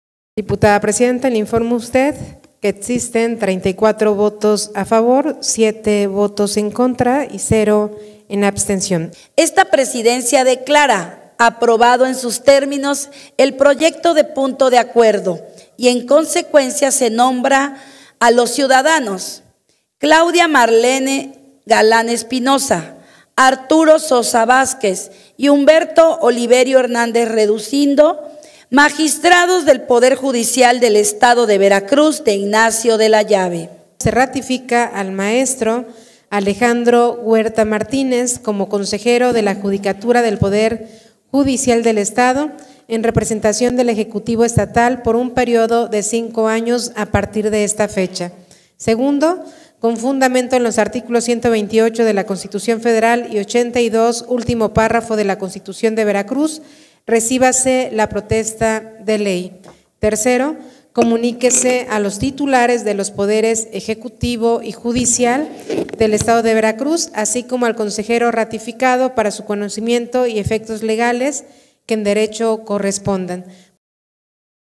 Congreso del Estado nombra nuevos magistrados del PJEV
La LXVI Legislatura de Veracruz H. Congreso del Estado de Veracruz nombró nuevos magistrados del Poder Judicial del Estado de Veracruz y ratificó consejeros judiciales.

Las noticias breves confirmadas. 📰💻✔️